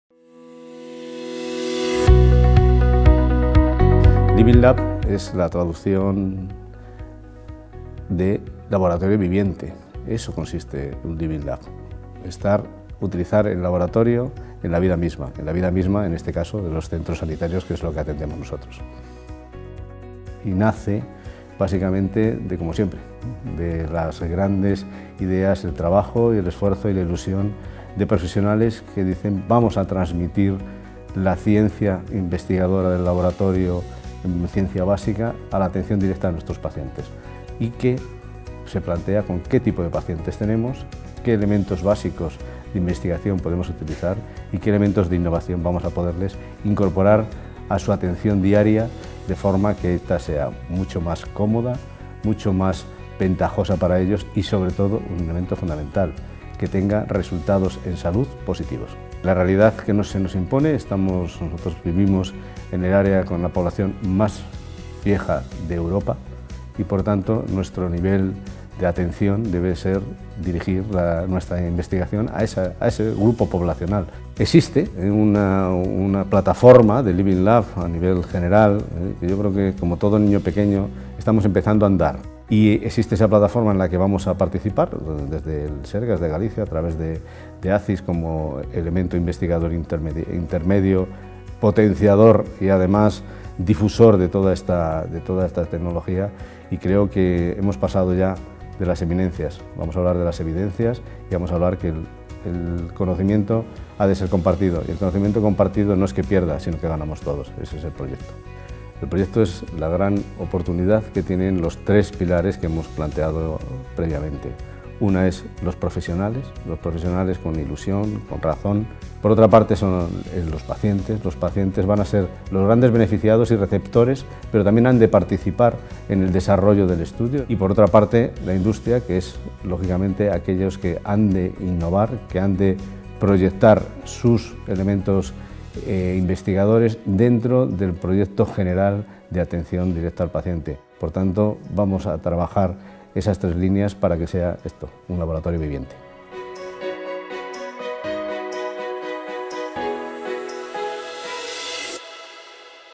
Proyecto Living Lab - zinkinn.es
Living Lab es una iniciativa impulsada por el Servicio Galego de Saúde y la Xerencia Xestión Integrada de Ourense que pone a disposición de las empresas un espacio físico en el Complexo Hospitalario Universitario de Ourense (CHUO). En este entorno se ha construido un espacio destinado al desarrollo de una hospitalización experimental que persigue introducir procesos de asistencia hospitalaria más accesibles, confortables, eficientes y seguros, mediante la definición de un modelo funcional innovador integrado en el proyecto y la dotación de equipos e instalaciones para diferentes escenarios Living Lab en los que se desarrollen estudios de investigación e innovación sanitaria.